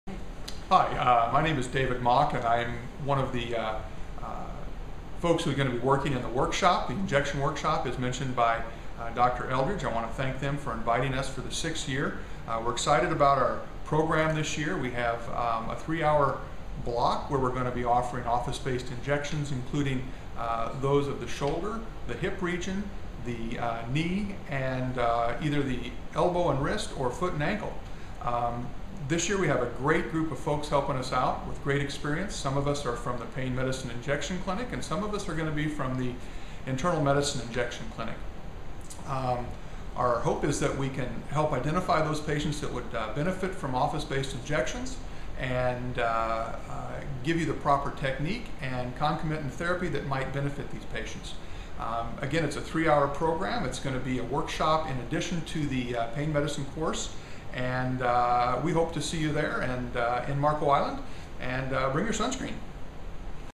Injection Workshop
David Mauck, M.D. from Mayo Clinic discusses the injection workshop being offered at the Pain Medicine for the Non-Pain Specialist course in Marco Island, FL.